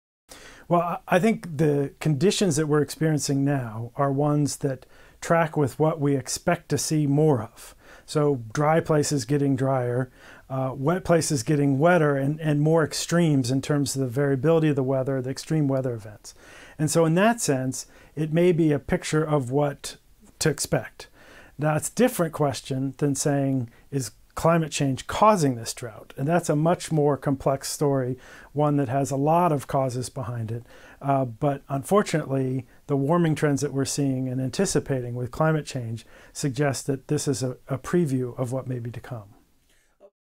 Sizzling Summer: Is This What Climate Change Feels Like?_part 2
In the wake of triple-digit heat and the sixth worst drought on record, many are wondering if this is what climate change feels like. With predictions suggesting this may be the new normal, we are also left to wonder about how prepared we are for increasingly extreme weather. Geoff Dabelko, Director of the Wilson Center's Environmental Change and Security Program, provides some perspective. (ref: 20120723 Dabelko 2)